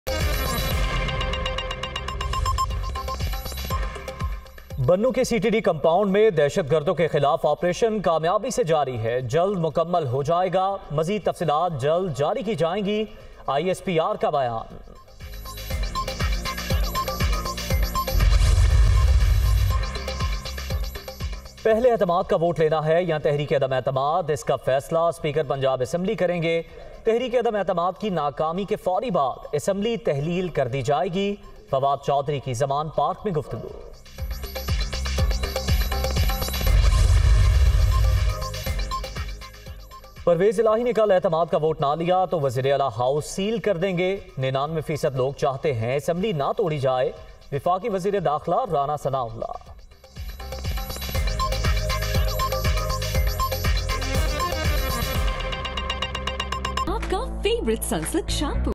Important Decision On Vote Of Confidence | Dunya News Headlines 07:00 PM | 20 December 2022
Can CM Parvez Elahi 'regain trust' of assembly?

CM House will be sealed if Elahi fails to take vote of confidence: Rana Sanaullah